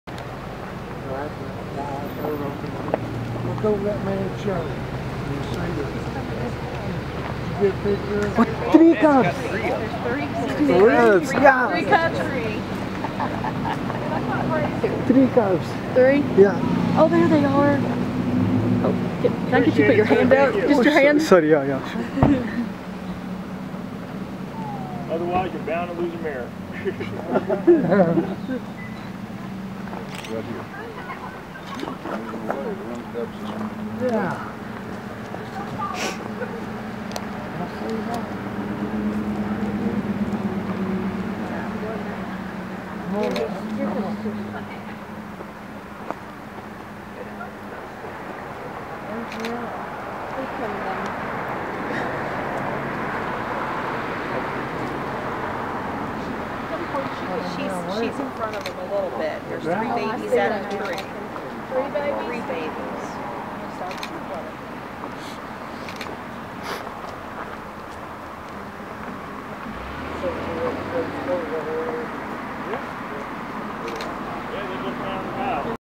Bear Cub Crossing
3 cute cubs playing around in Cades Cove, absolute fun watching them.